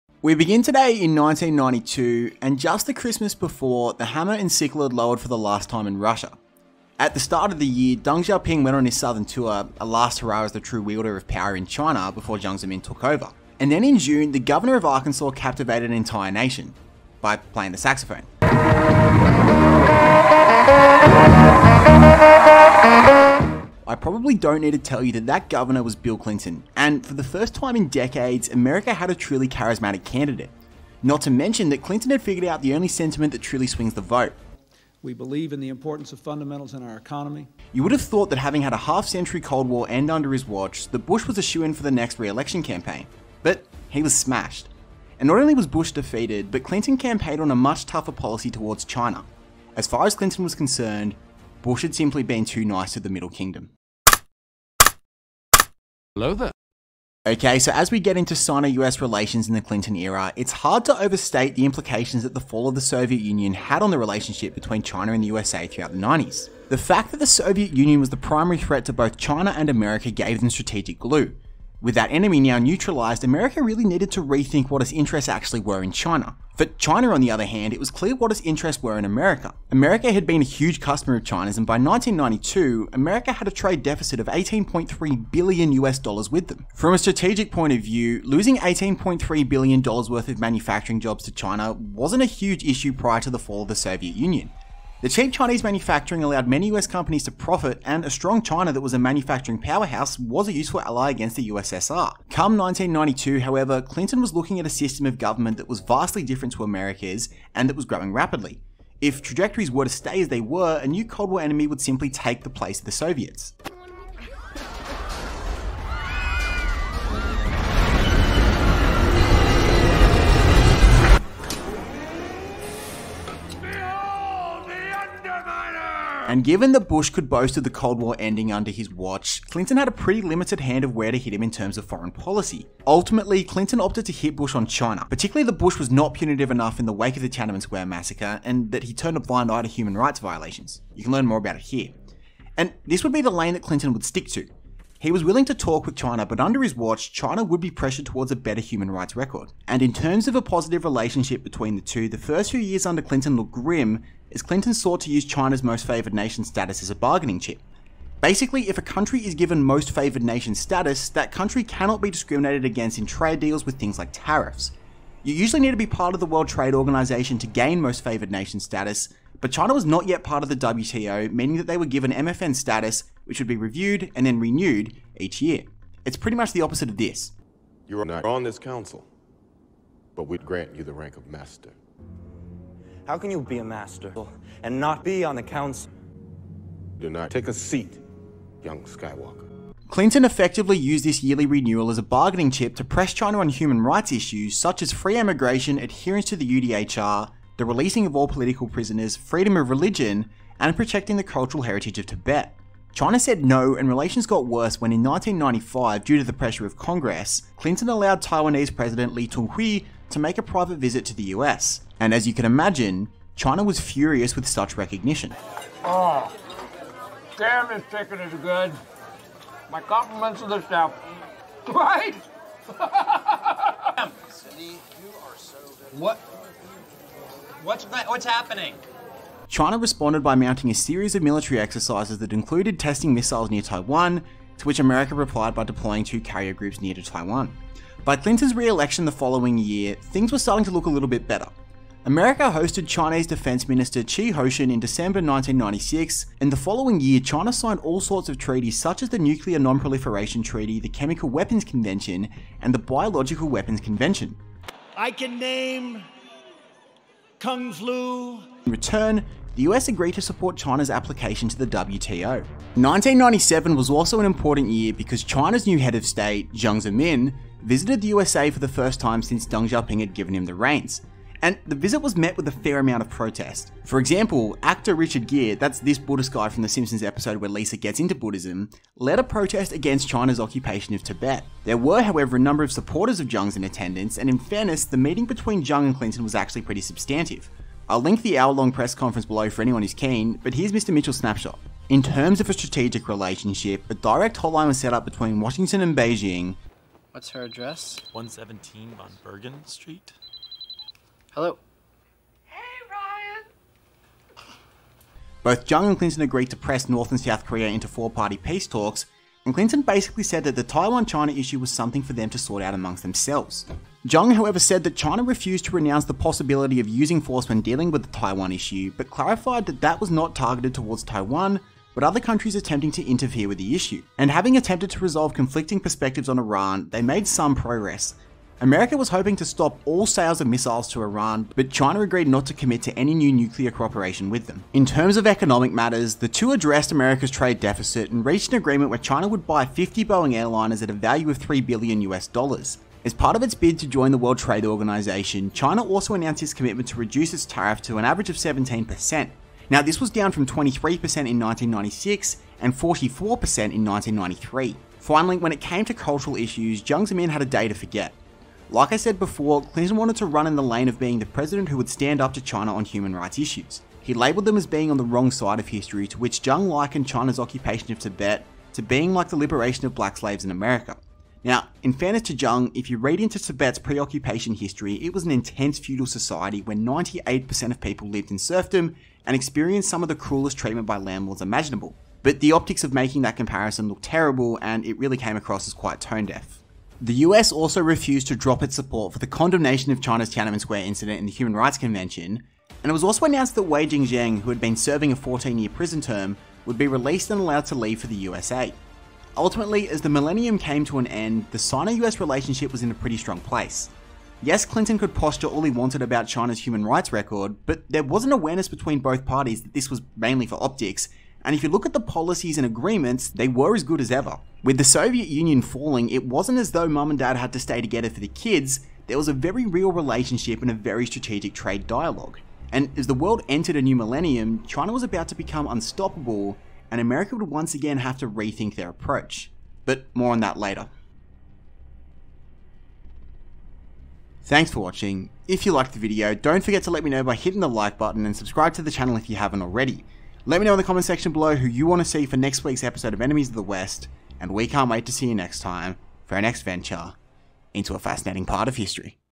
How did Bill Clinton manage a growing China?
With the fall of the Soviet Union in 1991, George HW Bush could boast of a foreign policy victory that no other president in half a century could; the end of the Cold War. Yet in 1992, the charismatic governor of Arkansas, Bill Clinton convicingly defeated the Republican. And as part of the election campaign Bill Clinton criticised his predecessor on his approach to China; mainly that he was too soft of them in the wake of the Tiananmen Square massacre.

Upon taking office, Bill Clinton pressed China on their human rights record and attempted to use their status as "Most-Favoured Nation' to leverage them. This was promptly rejected and tensions increased in the following years as Taiwanese President, Lee Tung-Hui visited the USA.

However, in 1997, new leader of China, Jiang Zemin was due to visit the USA and serious diplomatic talks were about to take place. As Clinton and Jiang met, the two attempted to talk through an approach that would take both nations into the 21st century.